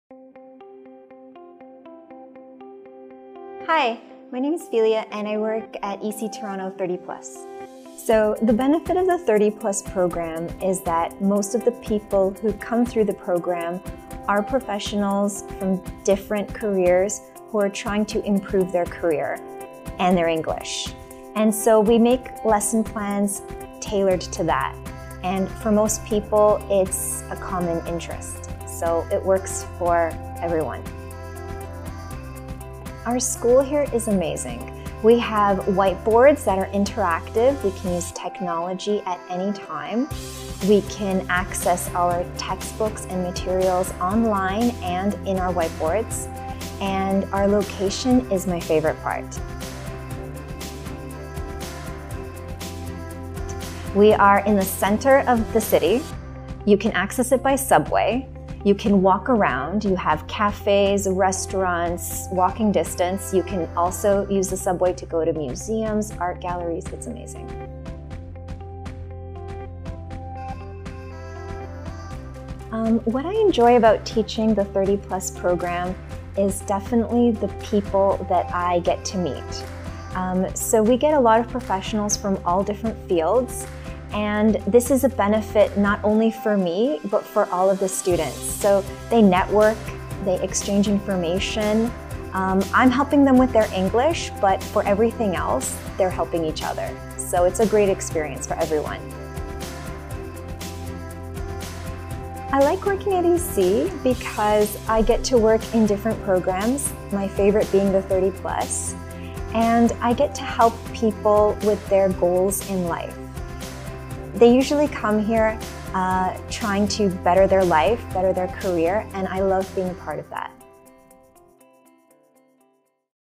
Inglés para mayores de 30 años - EC Toronto 30+
El programa 30+ está destinado exclusivamente a estudiantes más maduros que busquen una experiencia de inmersión lingüística que les suponga un reto y les ayude a alcanzar sus metas de aprendizaje. Toronto es la ciudad más grande y bohemia de Canadá y el lugar ideal para conocer estudiantes de tu edad y dominar el inglés de una vez por todas.